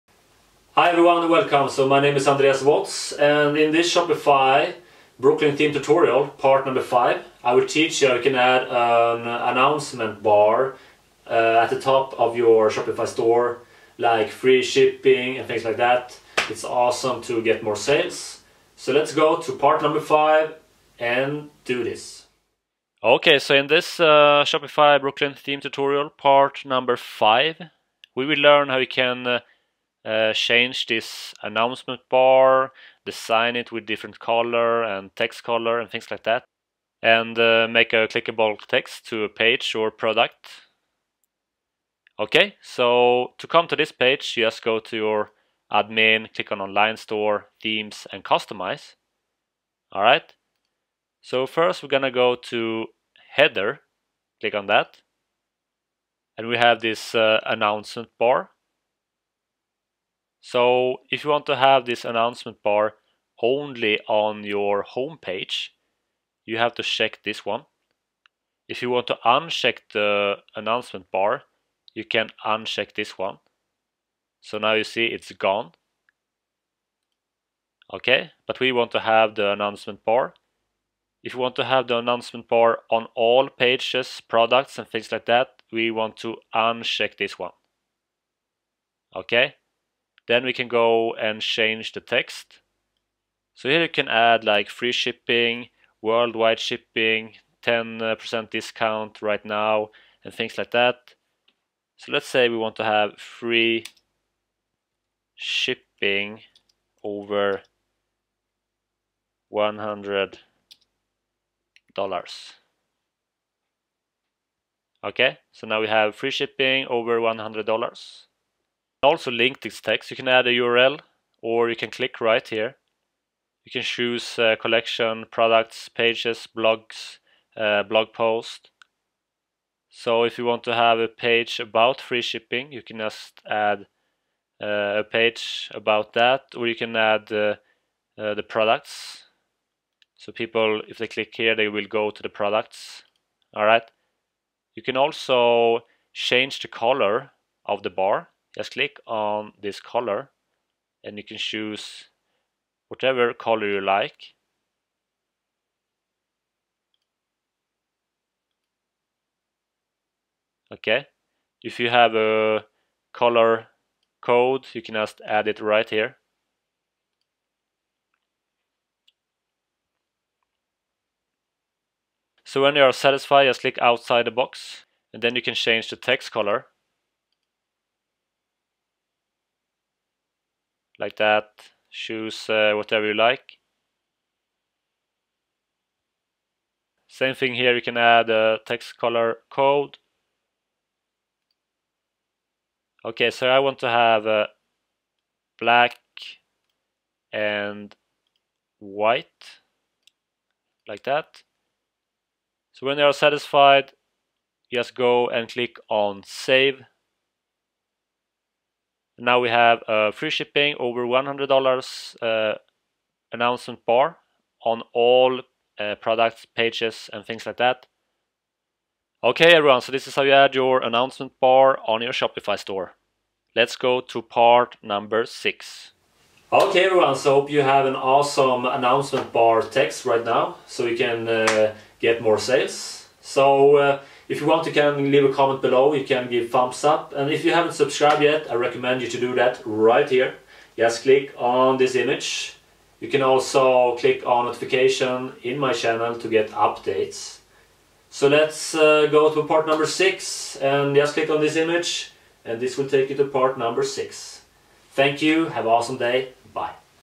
Shopify Brooklyn Theme Tutorial (Part 5) - How to Add Announcement Bar on Shopify 2018
In this Shopify Brooklyn theme tutorial part 5, I will teach you how you can add an announcement bar on your Shopify store to get your message out about promotion and stuff like that.

This video is perfect for you who wants to know how to make a Shopify store for beginners.

FOLLOW ME

Snapchat. Andreas Waatz